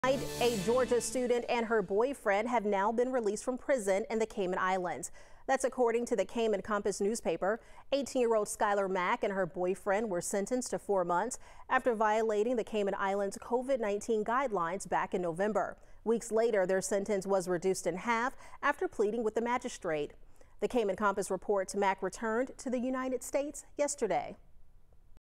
Georgia native, boyfriend released from prison in Cayman Islands
18-year-old Skylar Mack and her boyfriend were sentenced to jail time after breaking COVID guidelines in November.